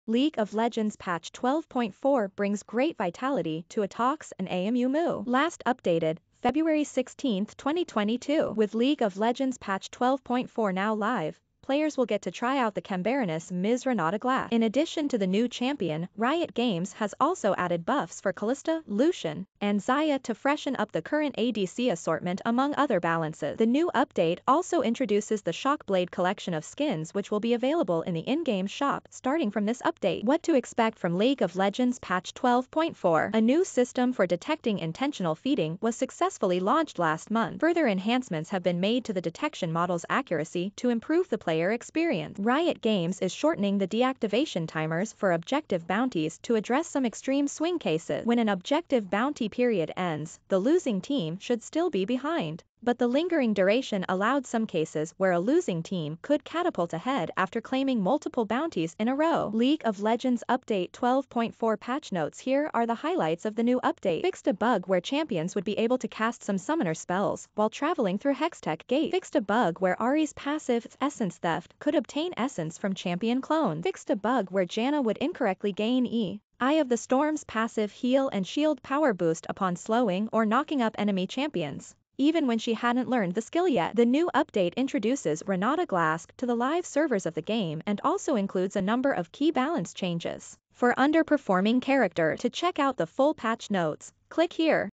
League of Legends Patch 12.4 brings great vitality to Attox and Amu Mu.
but the lingering duration allowed some cases where a losing team could catapult ahead after claiming multiple bounties in a row. League of Legends Update 12.4 Patch Notes Here are the highlights of the new update even when she hadn’t learned the skill yet The new update introduces Renata Glasc to the live servers of the game and also includes a number of key balance changes for underperforming characters. To check out the full patch notes and Xayah to freshen up the current ADC assortment among other balances. The new update also introduces the Shockblade collection of skins which will be available in the in-game shop starting from this update. What to Expect From League of Legends Patch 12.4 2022 With League of Legends’ patch 12.4 now live A new system for detecting intentional feeding was successfully launched last month. Further enhancements have been made to the detection model’s accuracy to improve the player experience. Riot Games is shortening the deactivation timers for Objective Bounties to address some extreme swing cases. When an Objective Bounty period ends League of Legends Patch 12.4 brings great vitality to Attox and Amu Mu. Last Updated